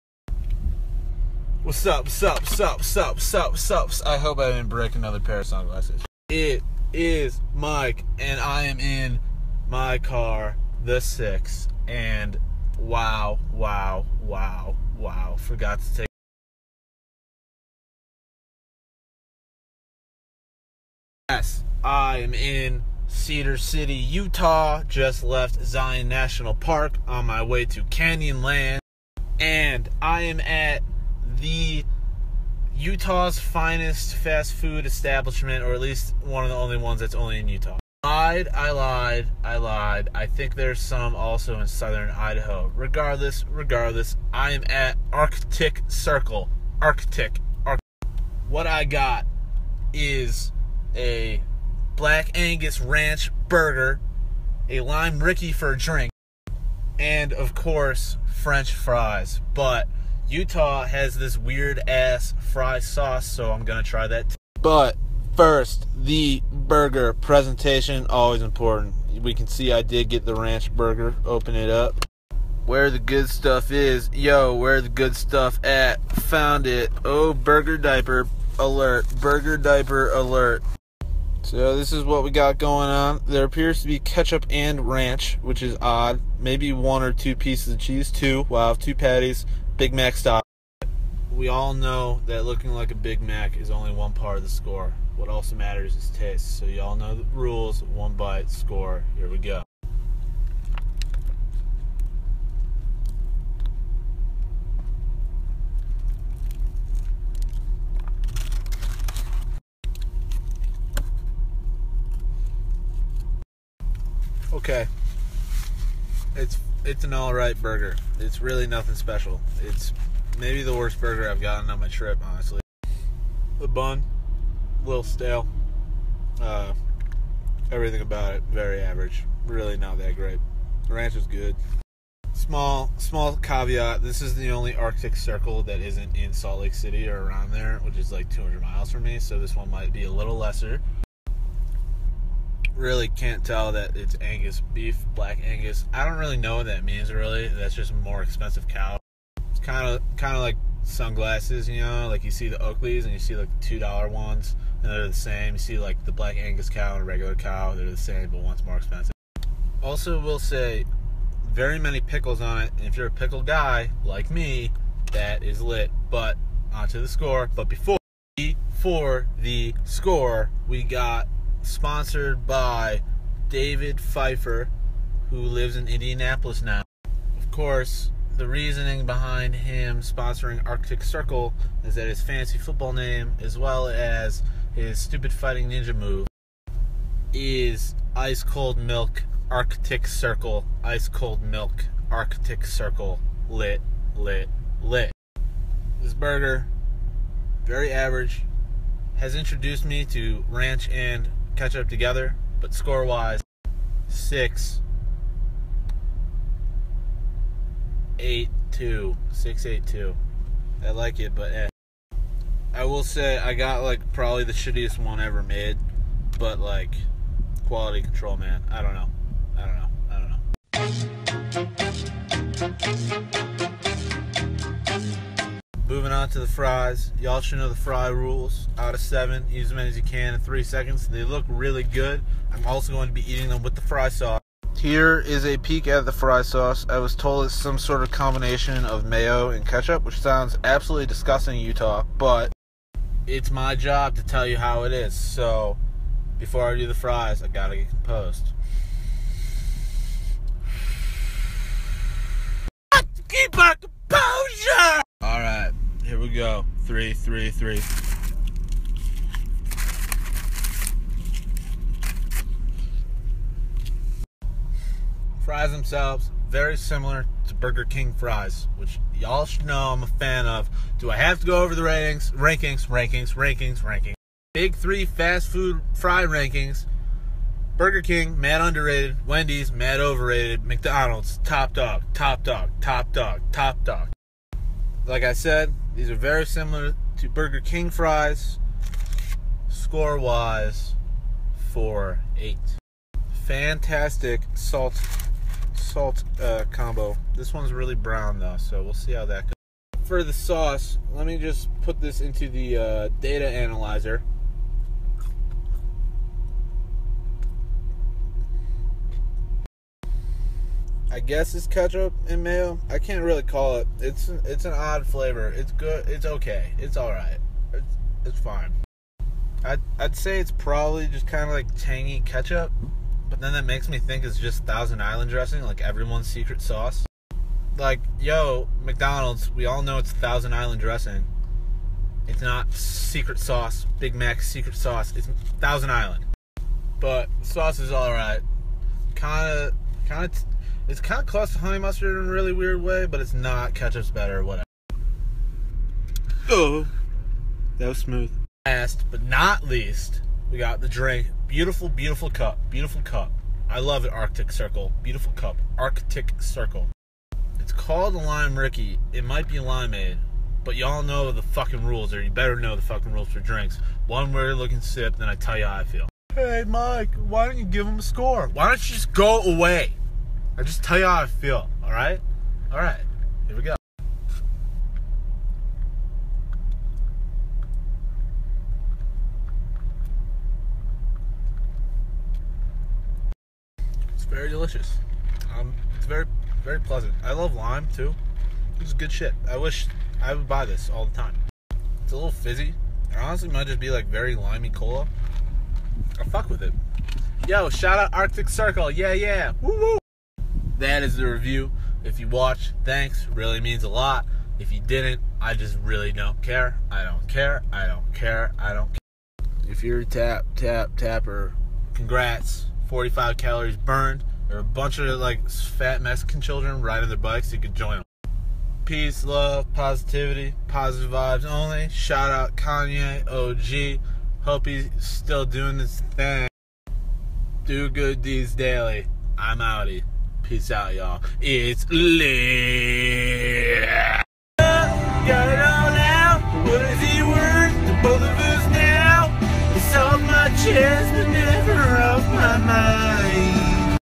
Arctic Circle Ranch Burger Review
I reviewed Arctic Circle's Ranch Burger, fries, fry sauce, and a lime rickey.

I post my reviews on snapchat and upload them to the tubes a couple days later so feel free to follow me @MikeMagz